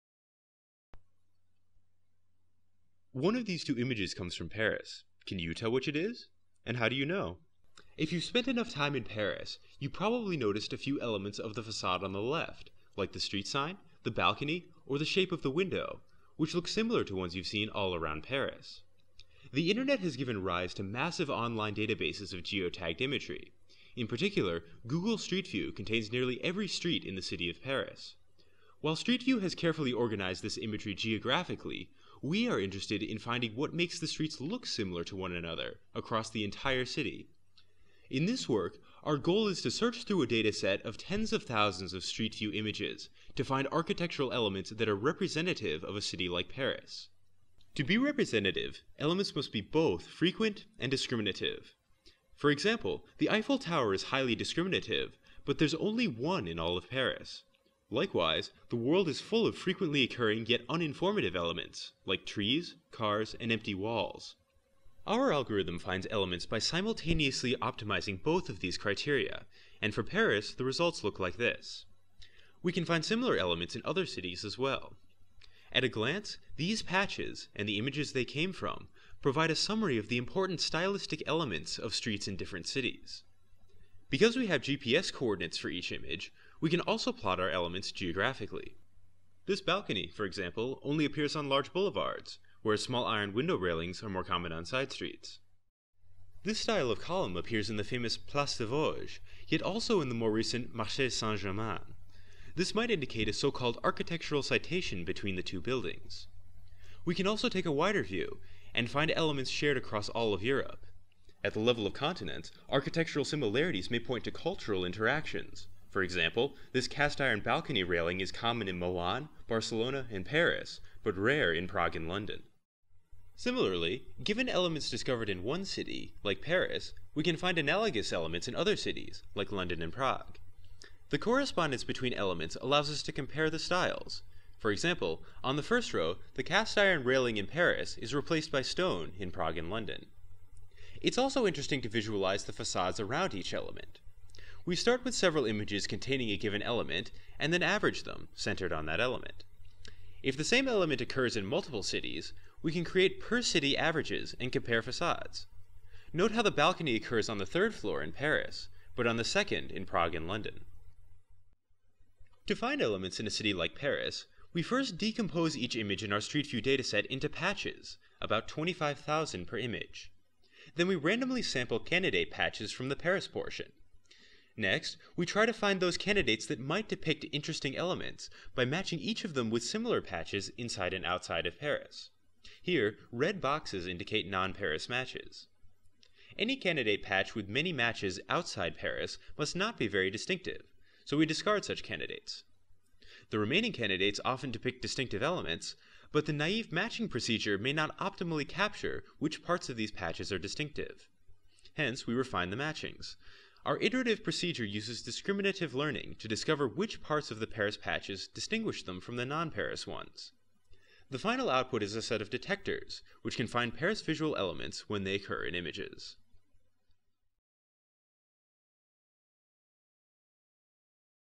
What Makes Paris Look like Paris?
Given a large repository of geotagged imagery provided by Google Street View, we can automatically find local visual elements, such as street signs, windows, or balconies, that are most distinctive for a given locale, such as the city of Paris.